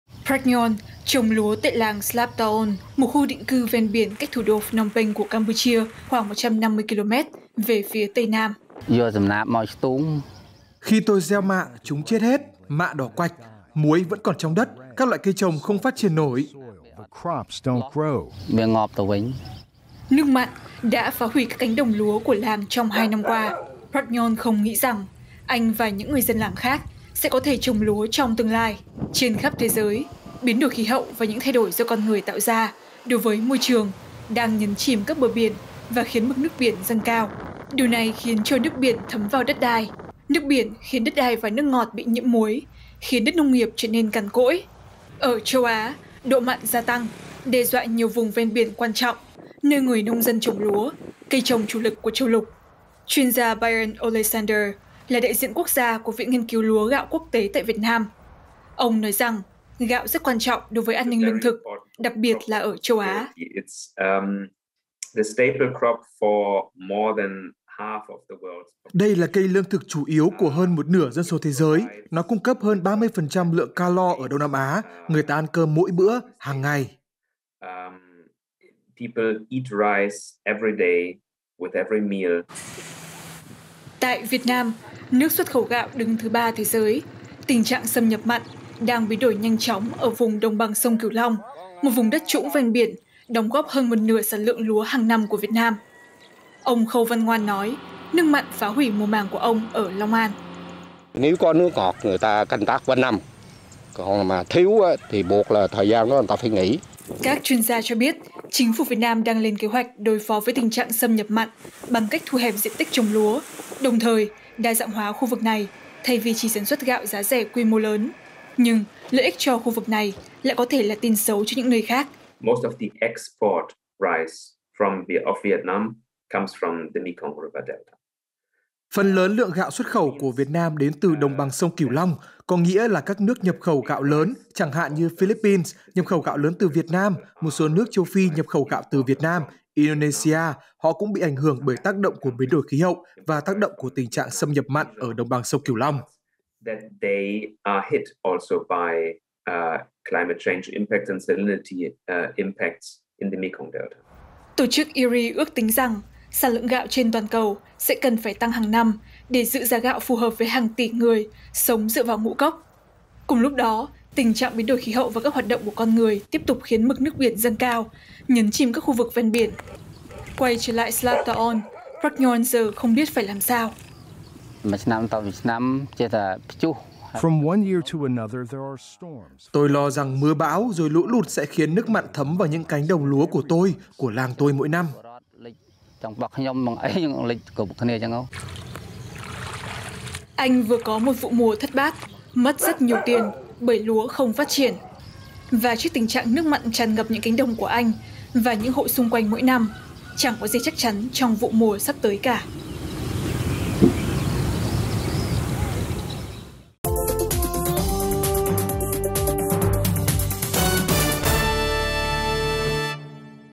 Tình trạng xâm nhập mặn đe dọa vựa lúa sông Mê Kông | VOA Tiếng Việt
Khắp nơi trên toàn thế giới, nước mặn ven biển đang ngày càng len lỏi sâu vào đất liền, làm ô nhiễm đất và nước với lượng muối đủ để giết chết mùa màng. Ở châu Á, nước mặn xâm nhập đang khiến một số nông dân, trong đó có ông Khâu Văn Ngoan ở Long An, gần như không thể trồng được loại cây lương thực chính của khu vực.

Worldwide, coastal saltwater is creeping further and further inland, tainting the land and water with enough salt to kill crops. In Asia, saltwater intrusion is making it nearly impossible for some farmers to grow the region’s staple food. Elise Cutts has more.